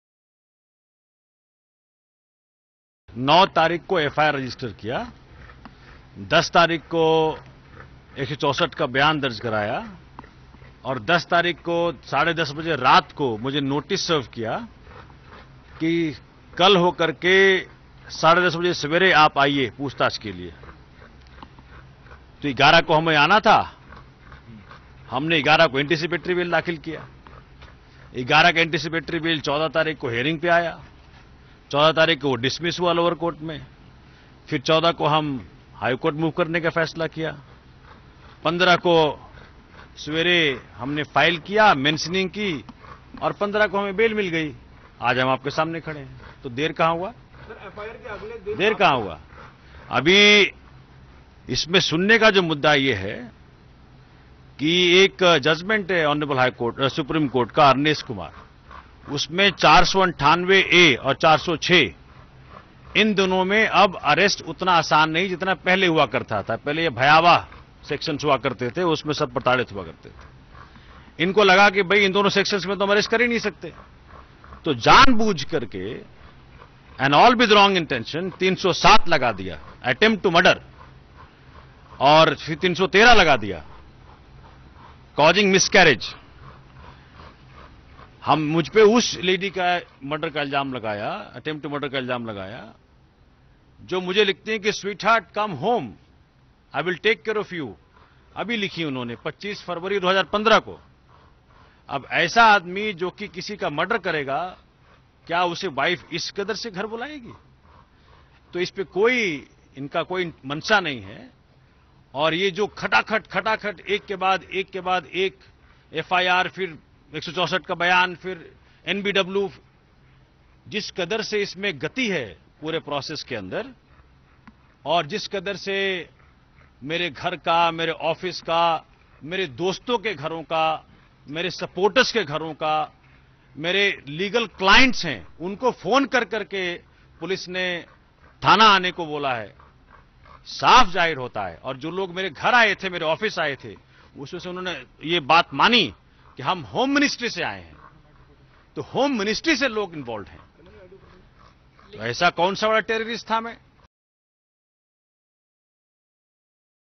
Somnath implicates MHA in domestic violence case against him (Part - 1)
New Delhi, Sep 16 (ANI): AAP MLA Somnath Bharti implicated Ministry of Home Affairs and central government in domestic violence case filed against him by his wife Lipika Mitra. Bharti elucidated that he was not absconding and only waited to get an interim relief. He also stated that the police looked for him as if he was a terrorist, which clearly shows the central government involvement. He also demonstrated the dog's non–responsiveness to him directing the do to bite anyone. Bharti got interim protection till Thursday and cannot be arrested, after his lawyer Vijay Agarwal seeked an anticipatory bail from Delhi High Court. Earlier a Delhi Court dismissed Bharti's anticipatory bail plea in the domestic violence case filed against him by his wife. Lipika Mitra, the wife of Somnath Bharti had filed a complaint with the Delhi Commission for Women (DCW) in June, accusing him of domestic violence and mental torture since 2010.